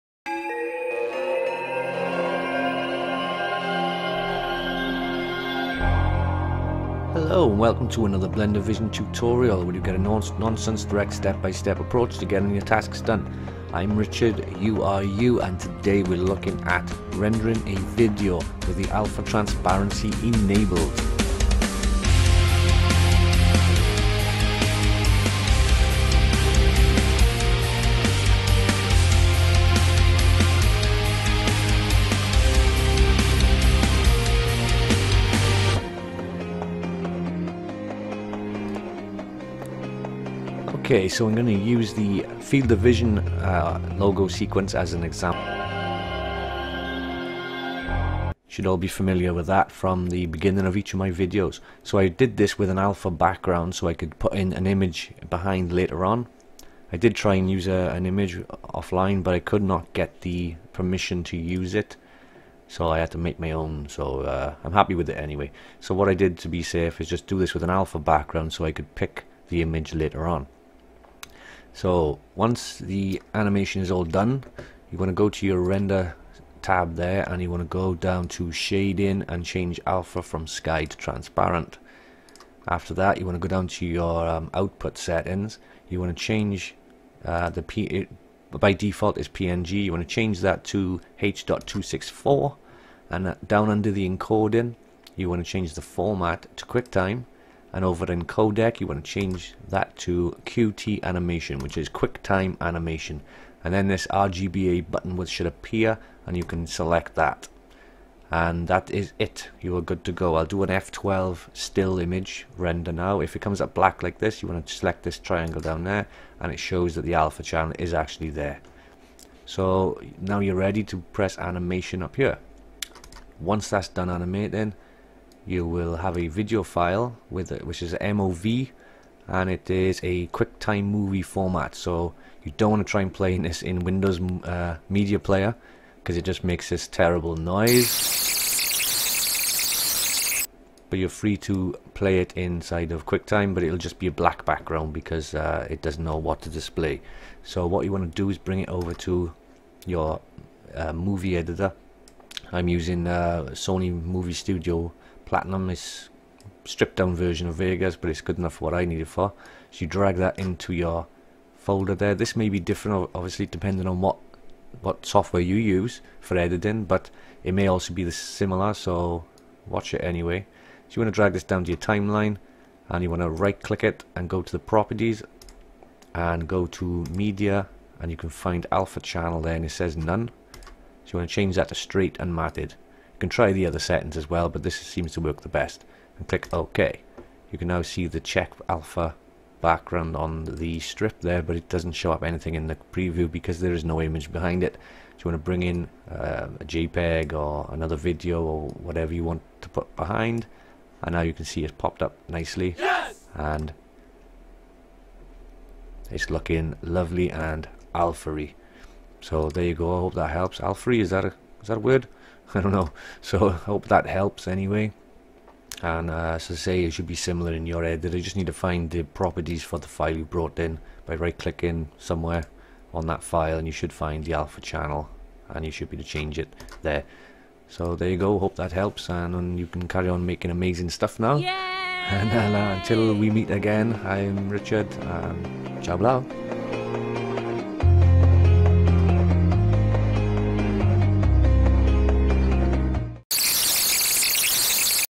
Blender - Rendering Videos With Alpha Transparency
A look at how to render transparency in videos.